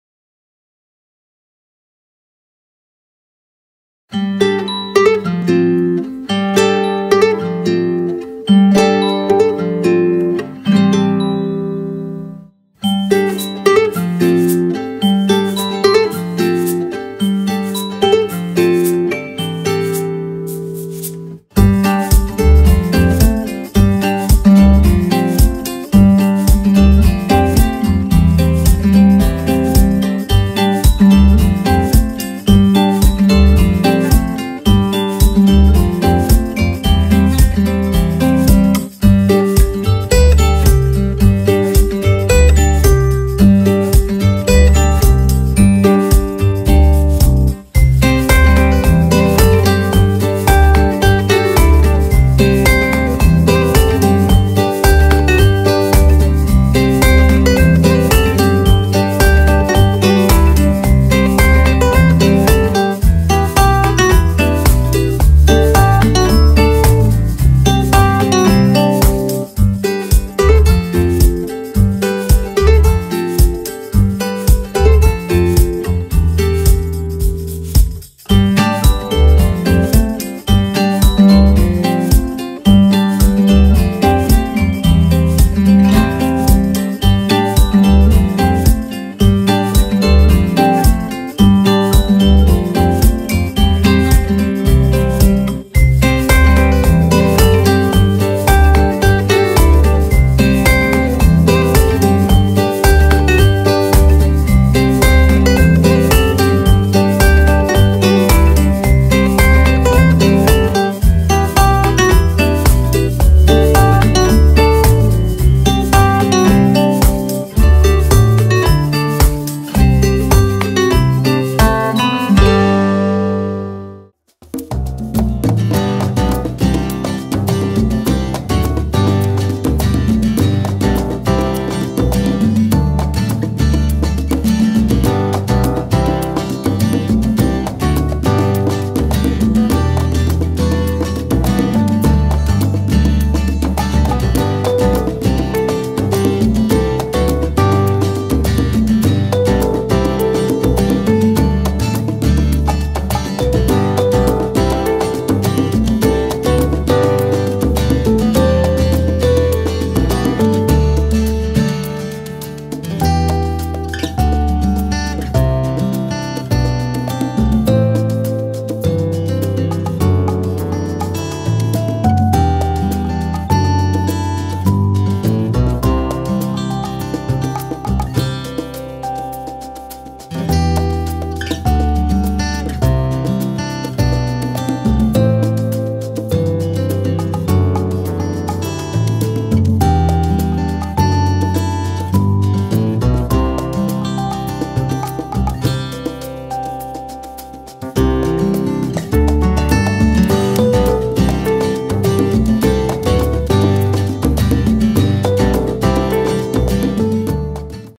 How To Make a Baby Owl With SculptCrete Paper Mache Clay
We are excited to introduce this new self-hardening paper mache clay to the market, super fun clay to use and sets up rock hard without waiting days for it to dry, it paintable in two hours or less. it's also great for super fine details.